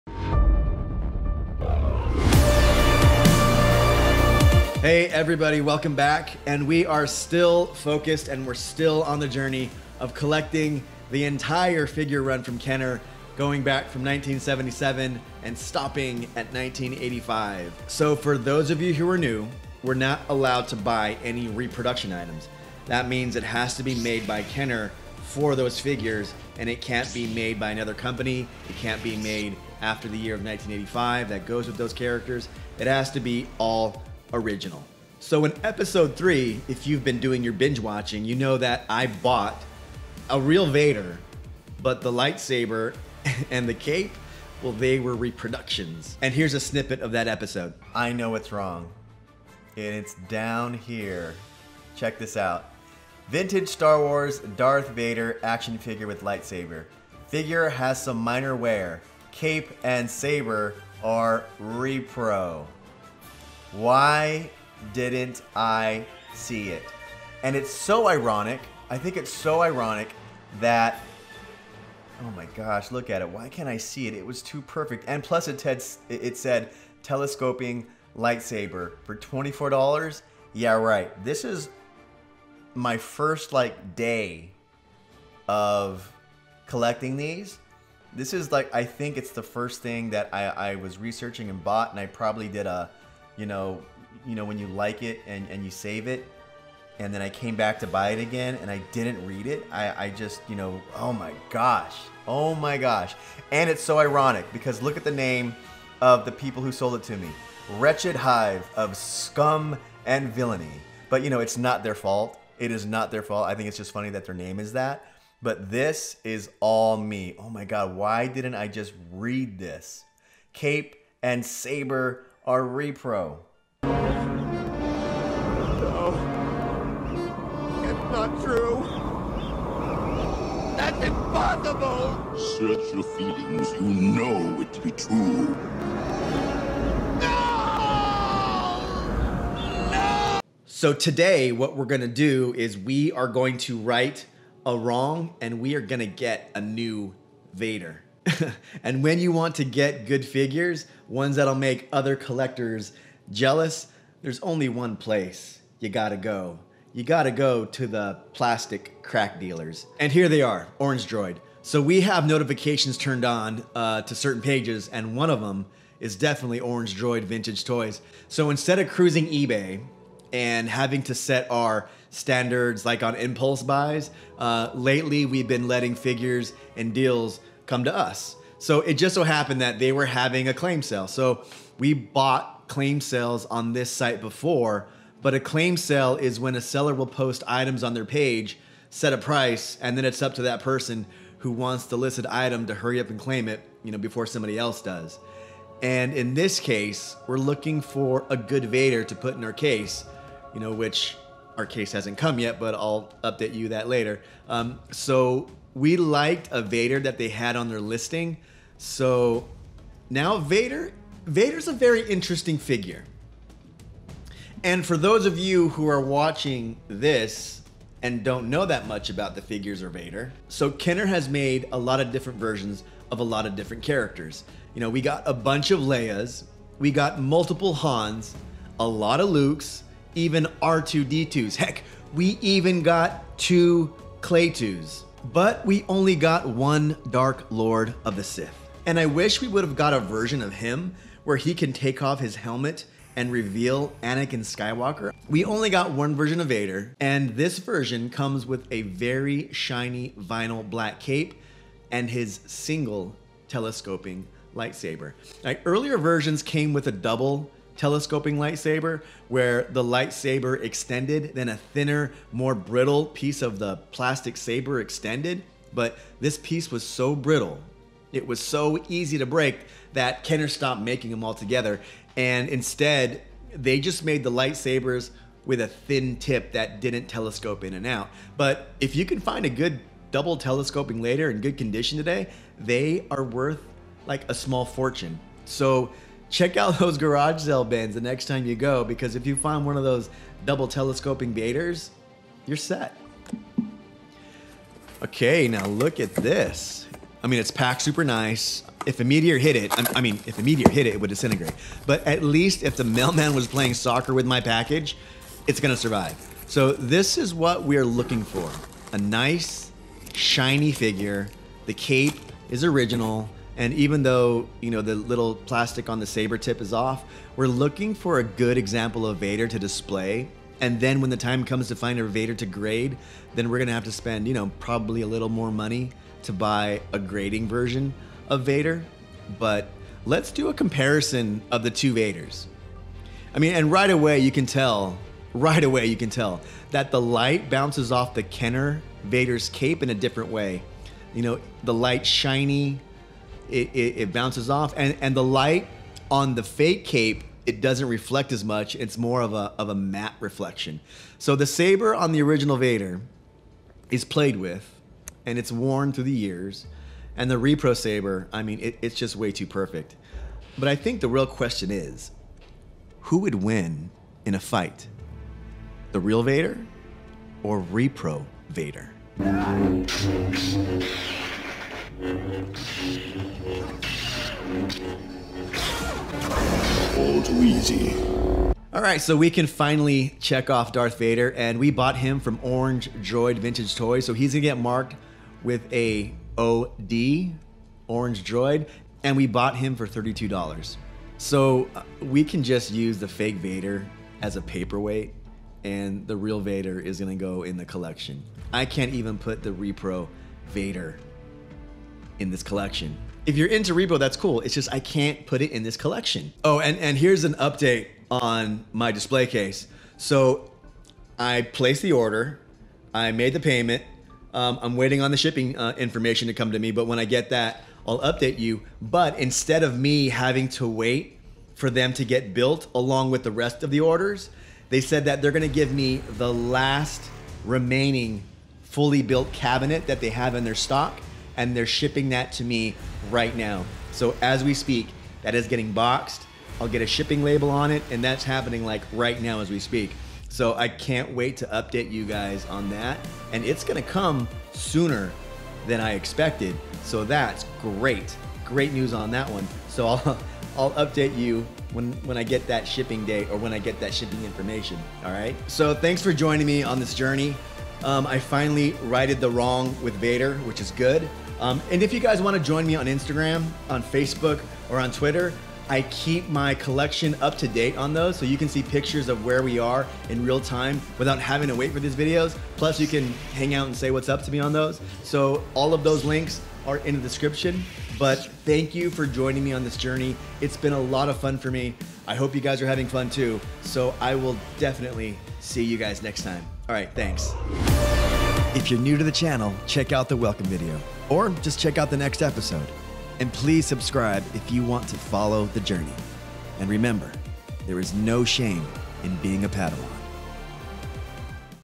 Repro Star Wars Toys & My Mistakes - EP 15
We found a reproduction vintage Star Wars Darth Vader action figure that we bought on eBay.  Spotting repro Star Wars toys is hard when you are buying online, but in this episode we try to find an authentic Darth Vader to replace the fake one to add to my collection.

For music that includes a global license for your content.
Legally and Financially cleared for your use.